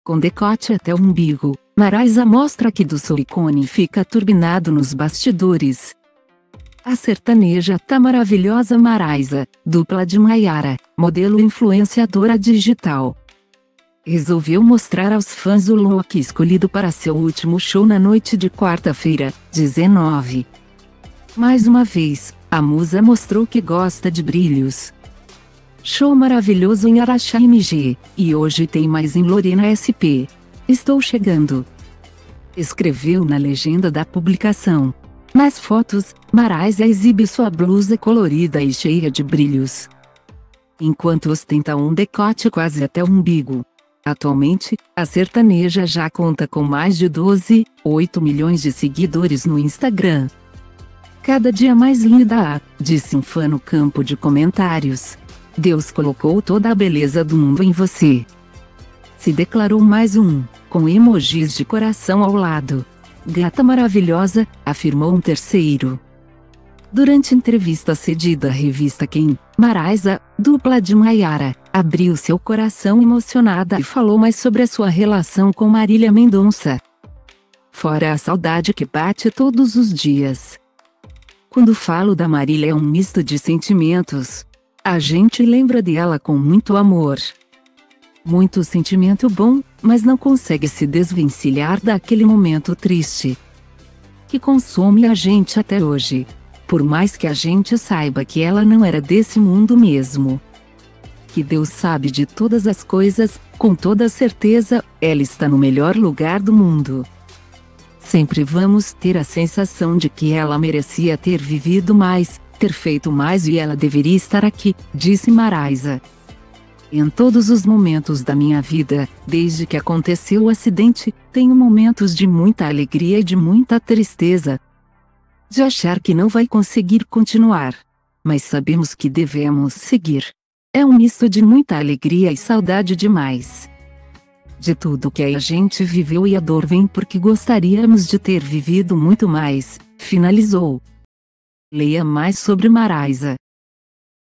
Com decote até o umbigo, Maraisa mostra que do silicone fica turbinado nos bastidores
A sertaneja tá maravilhosa! Maraisa, dupla de Maiara, modelo e influenciadora digital,resolveu mostrar aos fãs o look escolhido para seu último show na noite de quarta-feira (19).Mais uma vez, a musa mostrou que gosta de brilhos.